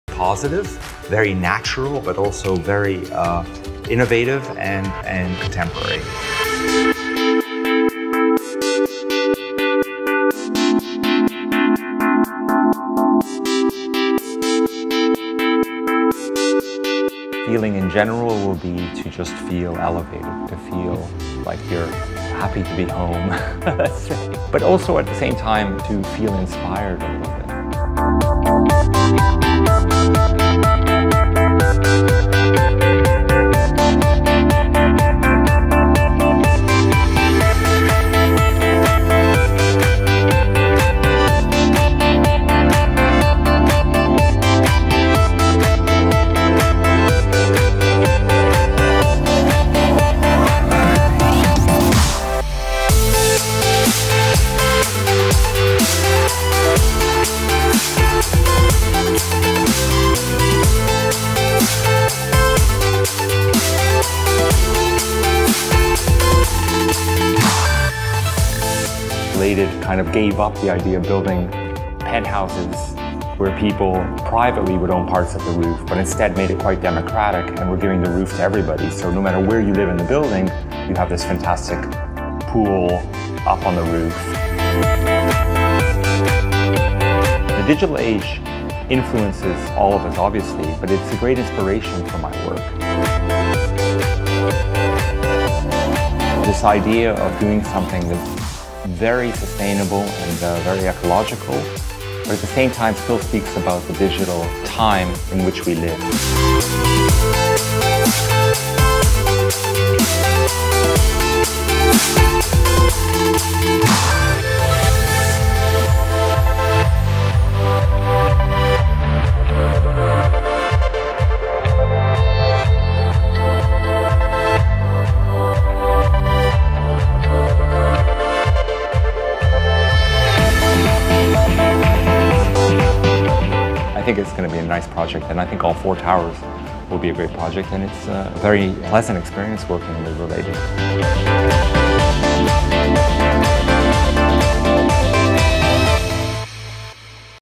Paraiso Miami - Hot New Community on The Bay
with 4 Buildings 98%  sold, The WiseCat is proud to announce our Developer Discounted Close-Out Pricing for 2016.  Get Wise!  Get WiseCat!!